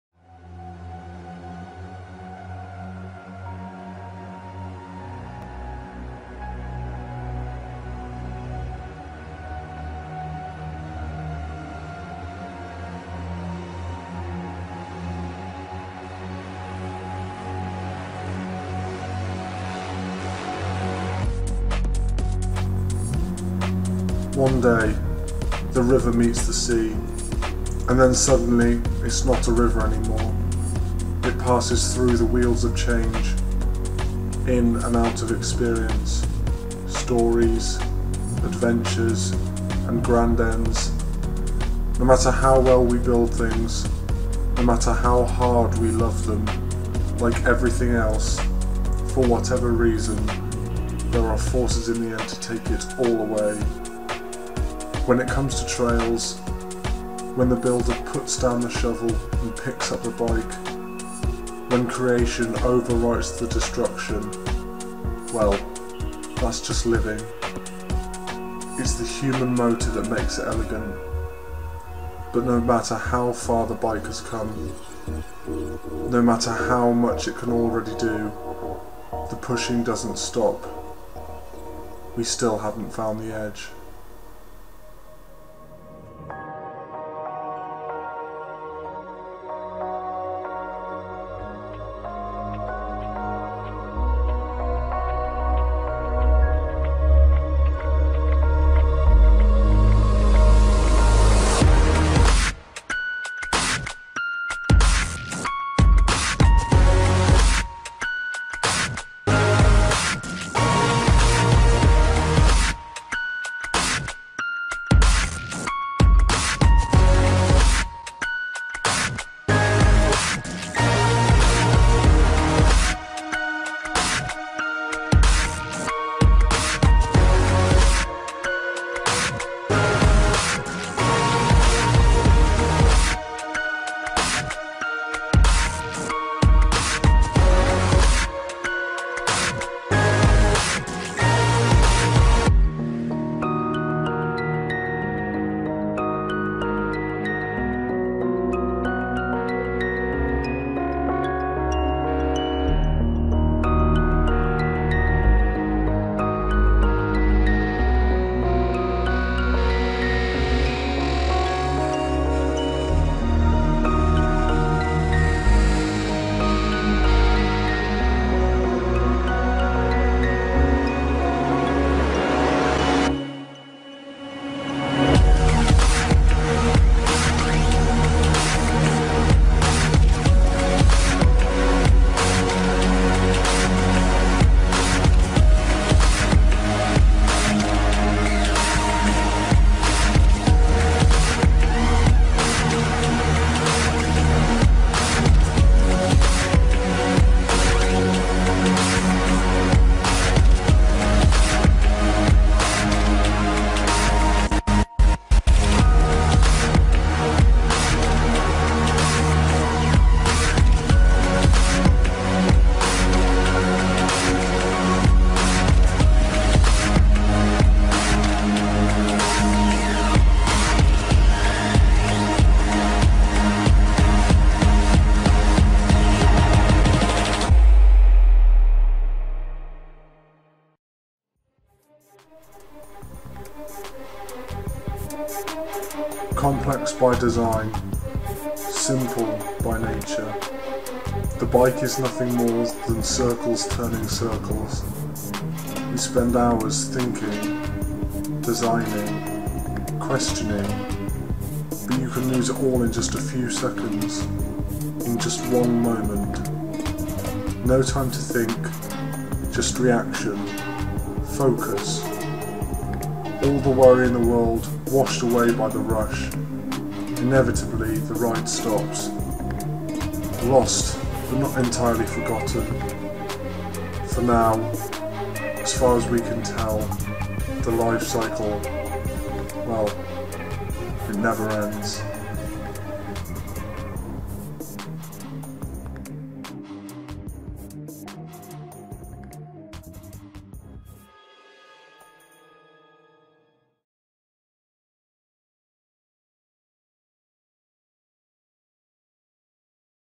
"Circles Turning Circles" Film Studies A Level Documentary
For my Film Studies A2 Level course we were given the task of creating a 5 minute film. I chose to create a documentary showing the sport of Downhill Biking. The final product gained full marks for my A Level coursework and I am sitting the exam on 21st June.

The main focus was to bring audiences who may have had no experience of a sport like this into the life of a downhill biker, showing them it's a lifestyle with risk but euphoric rewards.

It is probably obvious to a lot of people watching this who have previously seen videos of Biking documentaries that I took a lot of inspiration from the film "Life Cycles". It was my starting point but didn't define how I wanted to portray the documentary and made no impact on any of my original intentions. The ideas portrayed were already in the process of production before I had even done any research and watched Life Cycles.

On a final note, I want to say a huge thank you to Ben Church and Jony Smith who were my production team on this occasion and were perfect throughout the filming even in the cold, wet weather. Also to Dr. Giles Watson and Mr Henry Bew, my Film Studies teachers, for inspiring me to create something I am immensely proud of and for keeping the faith, even when I did keep my plans quiet throughout the year!